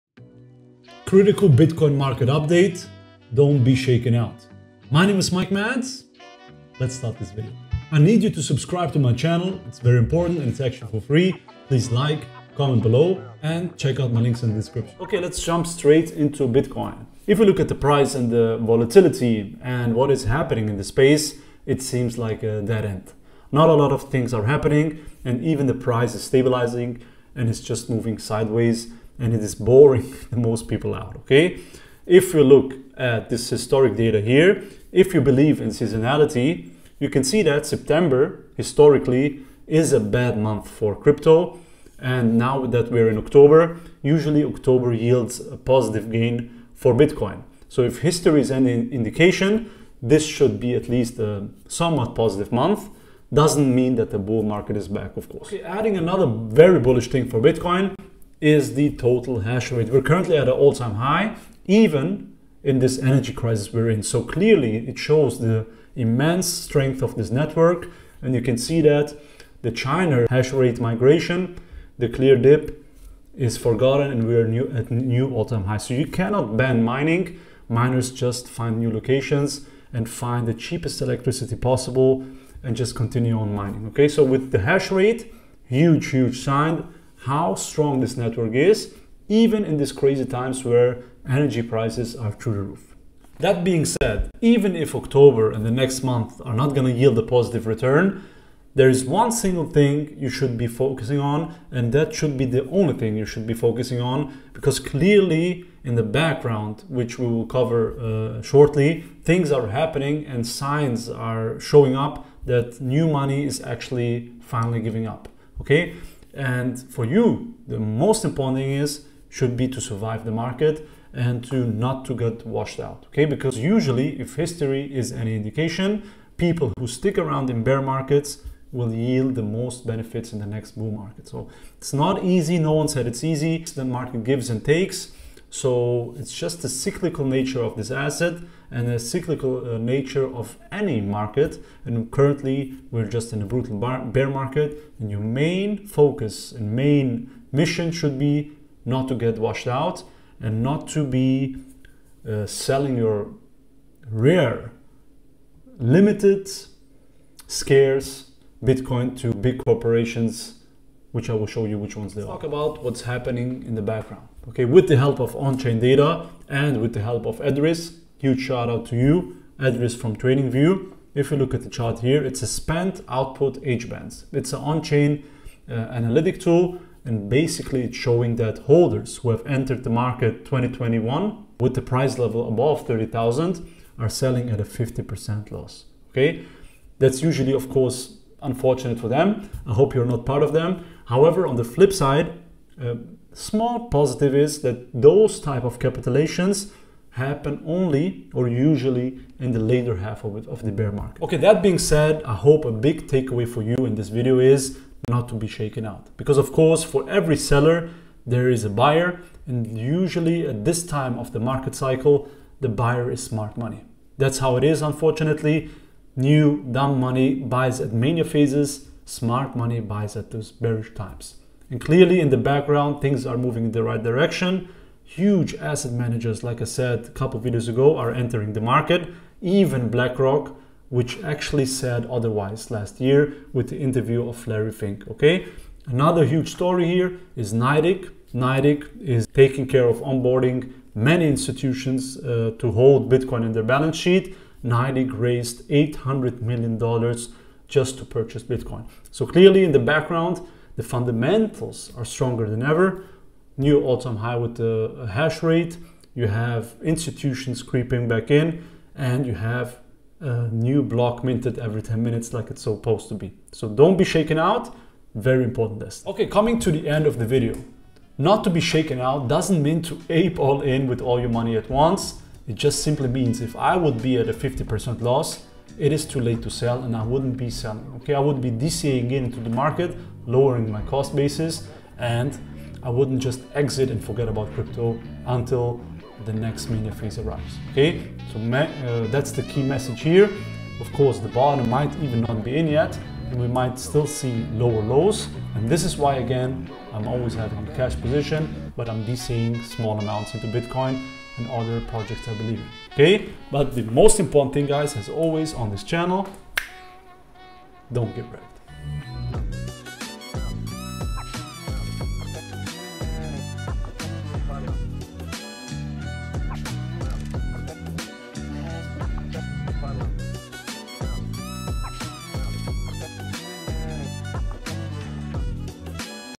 BITCOIN - AVOID THIS ONE URGENT MISTAKE 🚨
In this Bitcoin news video we over the bitcoin price the btc hashrate, the outlook for bitcoin in october and the importance of hodling in crypto!

🔻🔻🔻 MY RECOMMENDED CRYPTO EXCHANGE 🔻🔻🔻
to sign up for Crypto.com and we both get $25 USD